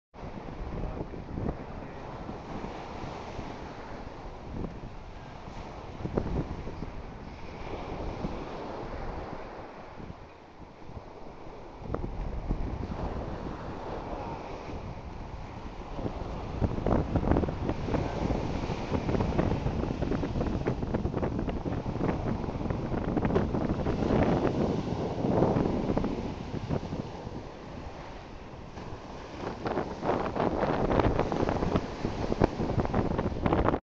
pelly film
Pelican riding the air currents on beach near Playa del Carmen